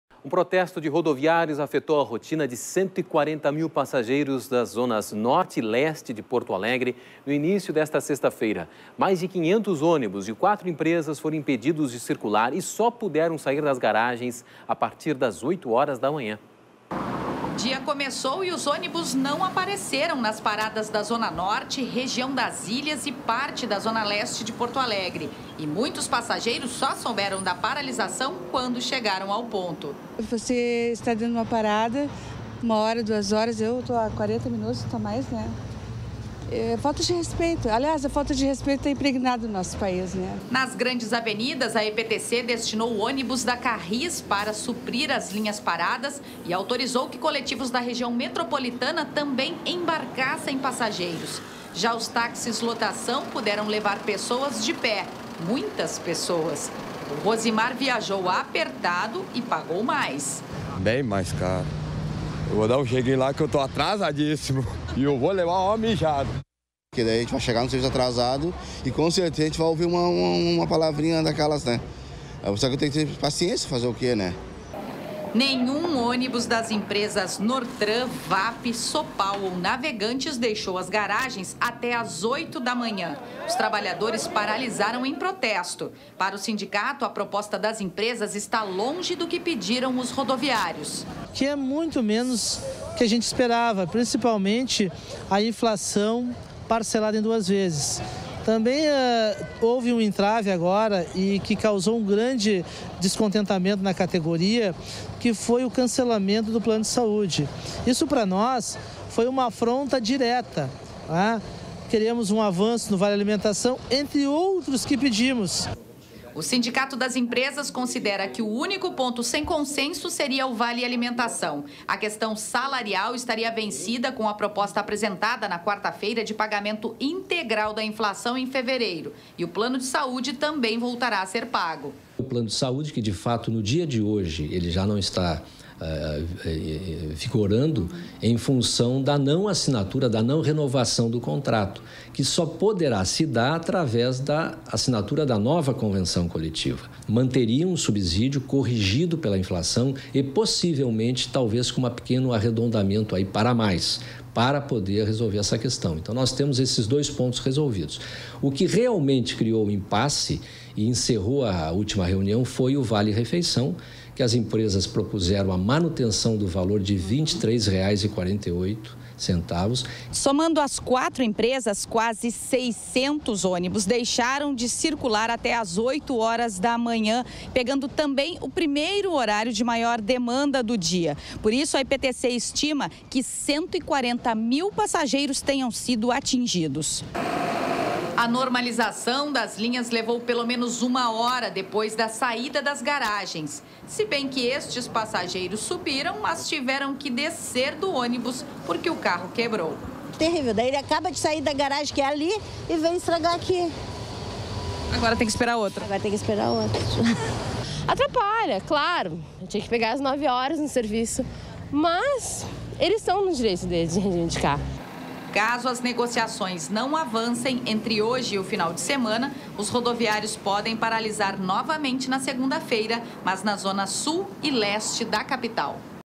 Descontentes, rodoviários protestam deixando milhares sem transporte público em Porto Alegre
Um protesto de rodoviários afetou a rotina de 140 mil passageiros das zonas norte e leste de Porto Alegre no início desta sexta-feira. Mais de 500 ônibus de 4 empresas foram impedidos de circular e só puderam sair das garagens a partir das 8h hoje pela da manhã.